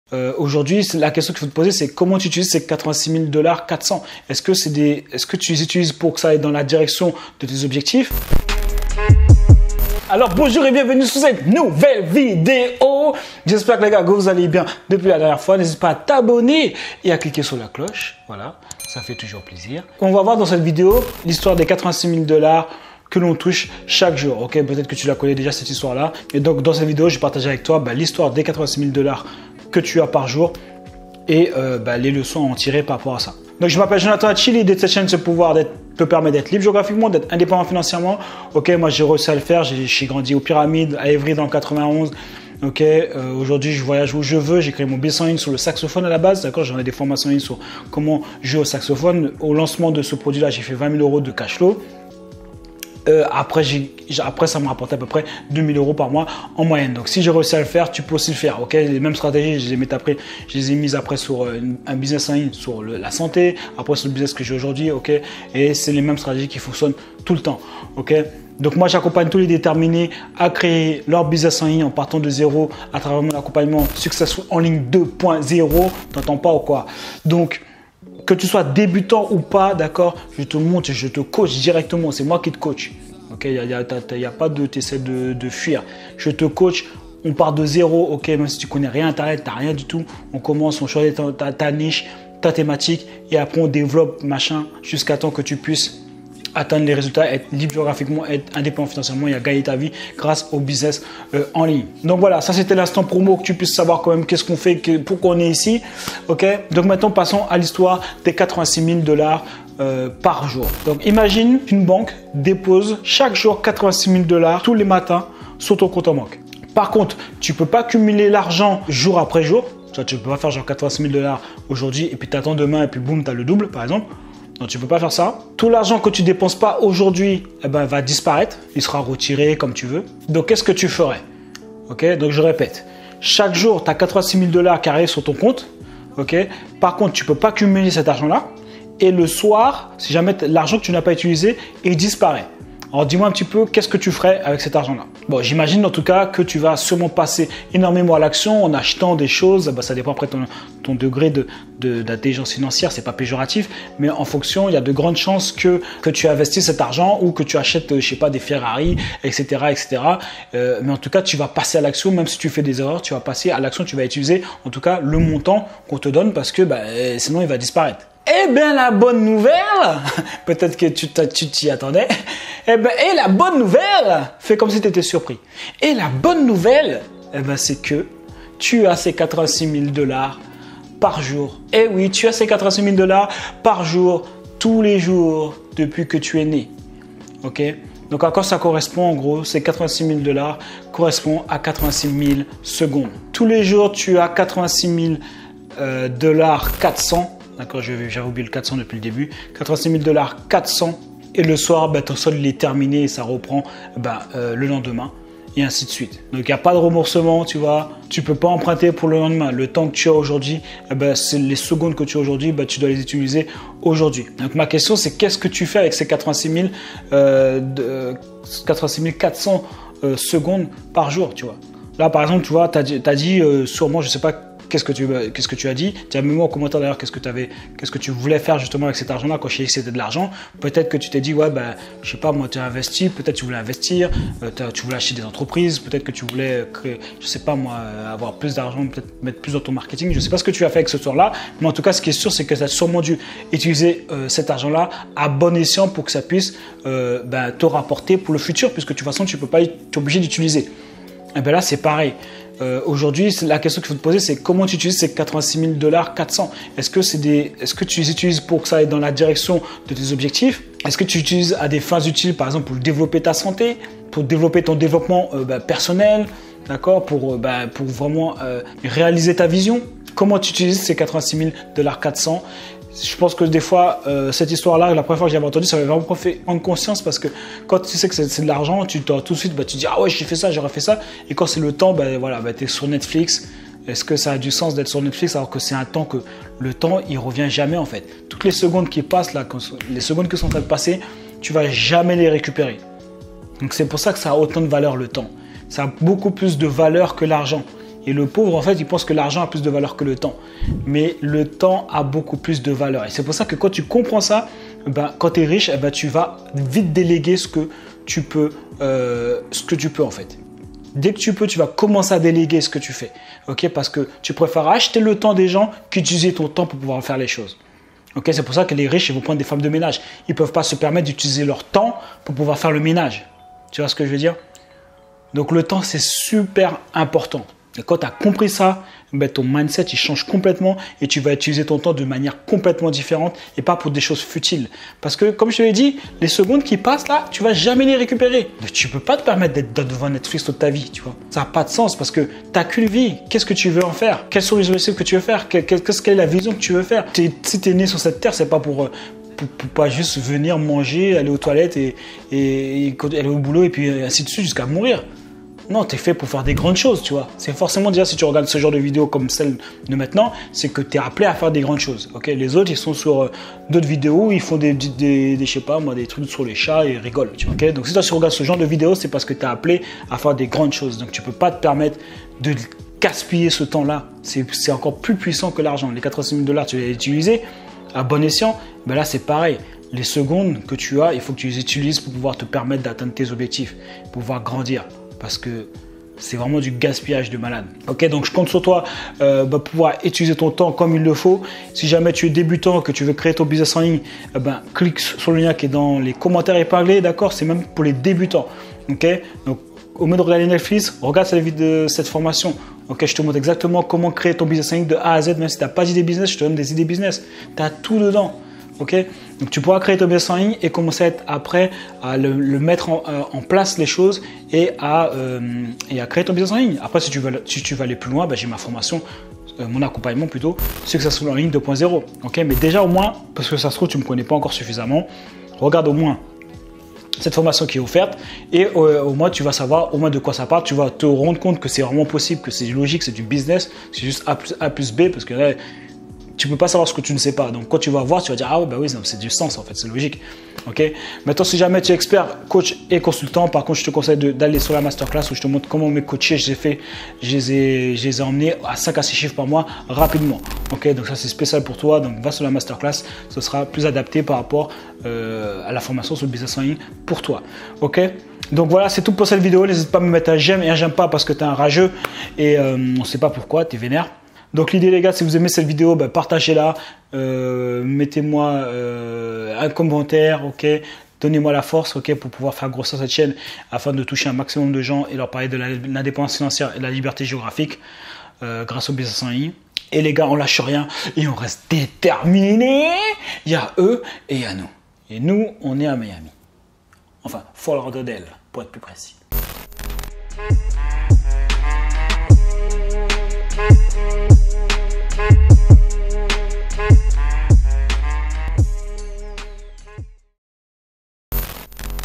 Booster sa productivité : L'histoire des 86 400 $ par jour
Bienvenue dans cette vidéo dans laquelle nous allons voir l'histoire des 86 400 $ par jour et comment obtenir sa liberté financière.
Imagine que ton compte bancaire soit crédité chaque jour de 86 400 $ par jour que tu es obligé de dépenser dans la journée même, que ferais-tu ?
La bonne nouvelle c'est que tu as ces 86 400 $ Tous les jours. En effet, tu as 86 400 secondes chaque jours. Les secondes que tu as aujourd'hui, tu dois les utiliser aujourd'hui, tu  ne peux pas les reporter à demain. La question c'est qu'est-ce que tu fais avec ces 86 400 $ par jour ? Le temps à beaucoup plus de valeur que l'argent !

Je te dis à toute de suite.

▬▬▬▬▬▬▬▬▬FORMATION OFFERTE▬▬▬▬▬▬▬▬▬▬▬

▬▬▬▬▬▬▬▬▬ MES COACHING ▬▬▬▬▬▬▬▬▬▬▬▬

▬▬▬▬▬▬▬▬ MES OUTILS ▬▬▬▬▬▬▬▬▬▬▬▬▬▬▬▬

▬▬▬▬▬▬▬▬▬▬▬▬ Témoignage client ▬▬▬▬▬▬

▬▬▬▬▬▬▬▬▬▬▬▬ LIENS ▬▬▬▬▬▬▬▬▬▬▬▬